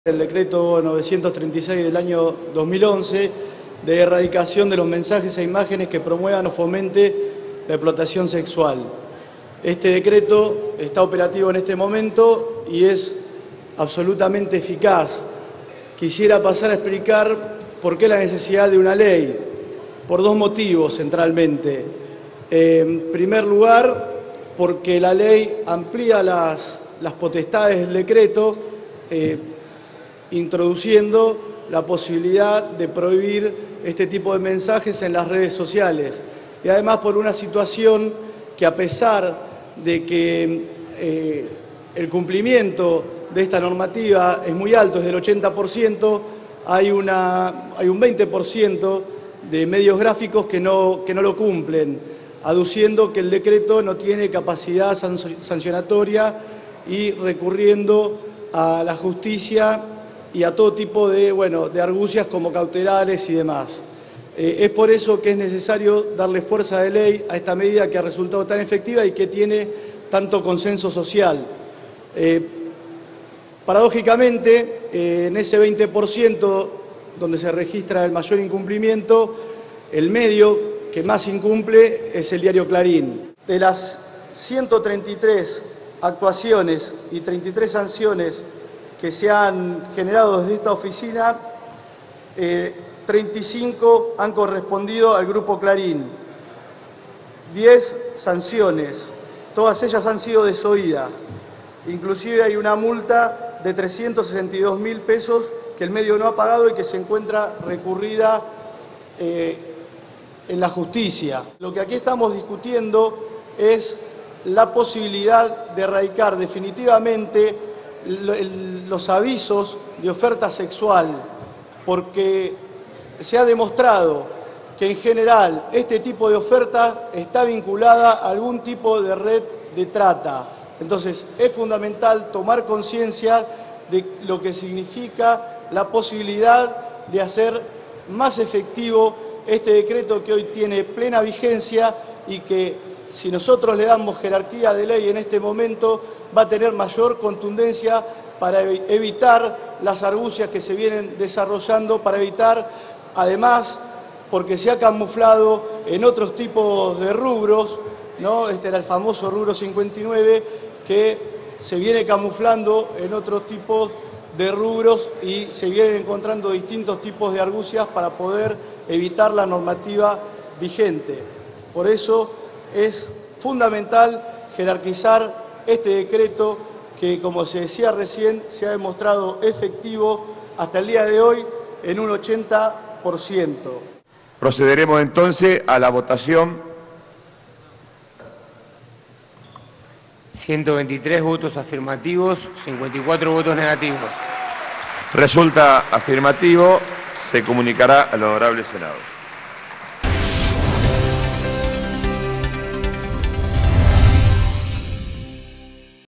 Diputados aprobó proyecto que prohíbe avisos que fomenten oferta sexual en medios de comunicación
La Cámara de Diputados aprobó y giró al Senado un proyecto de ley del diputado Andrés Larroque (Frente para la Victoria) por el cual prohíbe en cualquier medio de comunicación los avisos o anuncios que fomenten la oferta sexual y la trata de personas.

La iniciativa, que había recibido dictamen semanas atrás en el marco de un extenso debate, reunió 123 votos a favor, en tanto que 54 diputados se manifestaron en contra y 22 se abstuvieron.

El proyecto busca prohibir “los avisos, publicaciones, publicidades o cualquier otro tipo de mensajes que promuevan la oferta sexual o hagan explícita o implícita referencia a la solicitud de personas destinadas al comercio sexual, a través de cualquier medio de comunicación”, incluídas las redes sociales.

"Con este proyecto se busca jerarquizar el decreto 936 por el que se prohibieron en 2011 los anuncios que promuevan la oferta sexual y la trata y darle fuerza de ley", aseguró Larroque, al defender el proyecto en el recinto, donde precisó también que "el mayor infractor es el grupo Clarín, que se niega a cumplir con el decreto".

Y agregó: “Queremos reconocer que este proyecto se inspira en lucha de muchas organizaciones y personas y sobre todo en la de Susana Trimarco -madre de Marita Verón-.

“Estamos buscando erradicar definitivamente los avisos de oferta sexual, porque se ha demostrado que por lo general están vinculadas a red de trata”, dijo el secretario general de 'La Cámpora', para advertir a continuación que “muchos de los avisos que antes se conocían como Rubro 59 hoy se han camuflado en otros rubros para evitar ésta normativa”.

Desde la oposición, la UCR explicó su rechazo al proyecto en la voz de Manuel Garrido al señalar que “impulsa a la calle a las meretrices y las pone en riesgo de ser vulnerables a las redes de trata”; en tanto que su compañera de bloque, Patricia Giménez, dijo que “apelan a esta ley porque no les alcanza con el decreto para ponerle multas a Clarín”.

Desde el Frente Amplio Progresista, Aliciani Ciciliani también justificó el voto en contra al decir que "criminaliza a las víctimas" y que "en lugar de este proyecto, se debería reglamentar la ley de tratas".

De aprobarse, las sanciones para quienes incumplan con esta ley van de apercibimiento a multas de 60.000 a 3 millones de pesos, y podrían agravarse en caso de reiteración. El monto mínimo original de las multas era 300.000 pesos pero se bajó a pedido de los bloques opositores que prometieron su respaldo al proyecto.

Los diputados proponen conformar con ese dinero un fondo especial “destinado a la prevención, concientización y sensibilización en torno de los delitos de explotación sexual y trata de personas, así como de las diferentes formas de violencia contra las mujeres”.

En los fundamentos, la iniciativa precisa que la Oficina de Monitoreo de Publicación de Avisos de Oferta de Comercio Sexual, creada también por el decreto 2011, constató que "el 80 por ciento” de los medios de prensa gráfica supervisados dejaron esas publicaciones, pero advierte que aún hay “resistencias” a cumplir con ese mandato.

Esa oficina hizo referencia a que sólo en los primeros seis meses del año -entre enero y junio-, el diario Clarín publicó 16.552 avisos por los que obtuvo una ganancia de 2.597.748 pesos.